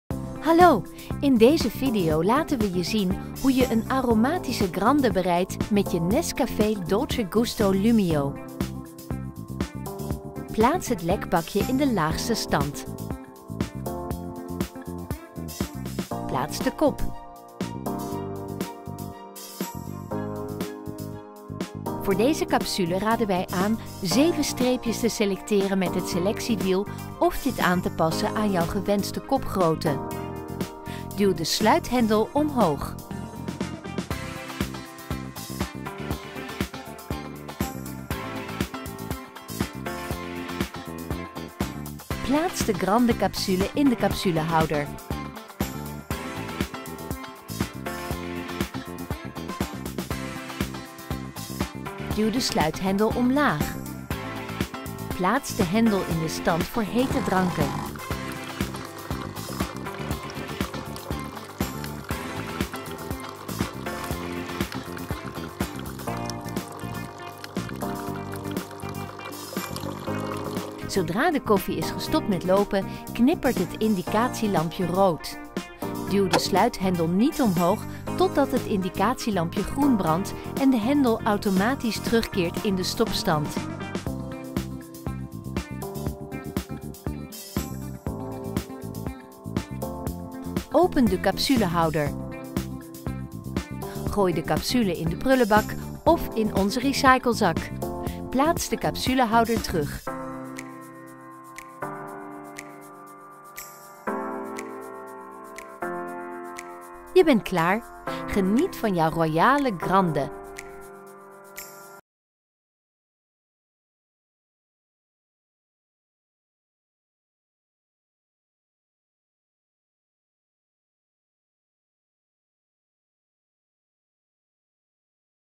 Grande Bereiden - Dolce Gusto® Lumio Koffiemachine
Ontdek hoe je een aromatische NESCAFÉ® Dolce Gusto® Grande kunt bereiden met de automatische Lumio koffiemachine van Krups®.
Relevante video’s:
Voor meer relevante informatie over NESCAFÉ® Dolce Gusto® bezoek onze
Wil je contact? Wij helpen je graag.